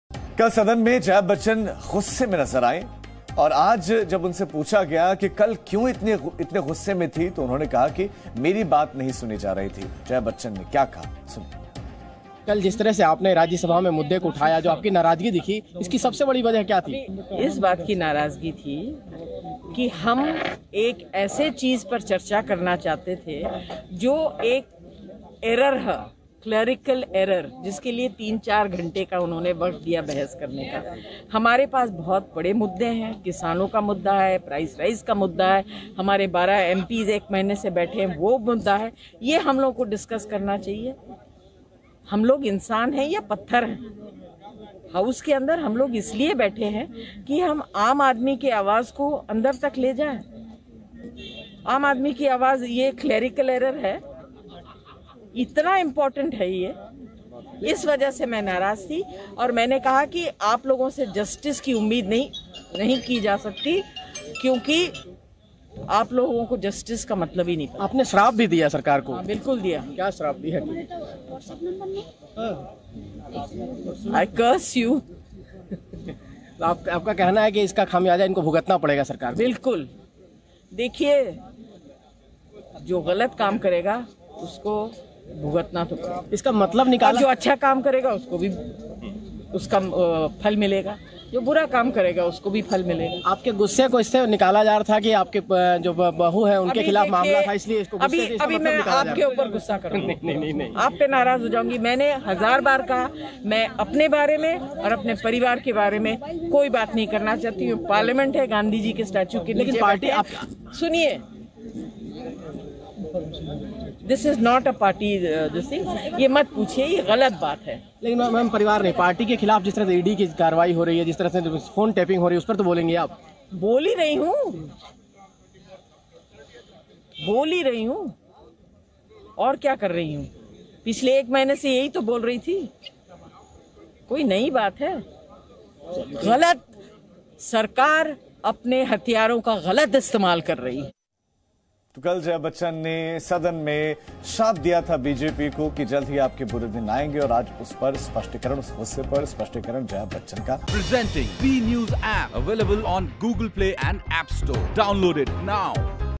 भुगतना तो जरूर पड़ेगा - किस पे फूटा जया का गुस्सा । Jaya Bachchan got angry in Parliament
Jaya Bachchan  got angry in Parliament: समाजवादी पार्टी से सांसद जया बच्चन ने आज एक बार फिर सरकार को आड़े हाथों लिया, जया बच्चन ने कहा कि बहस के लिए उन्हें समय नहीं दिया जा रहा था जिसकी वजह से वो नाराज हैं

Samajwadi Party MP Jaya Bachchan once again slammed the government, Jaya Bachchan said that she was not being given time for debate, due to which she is angry.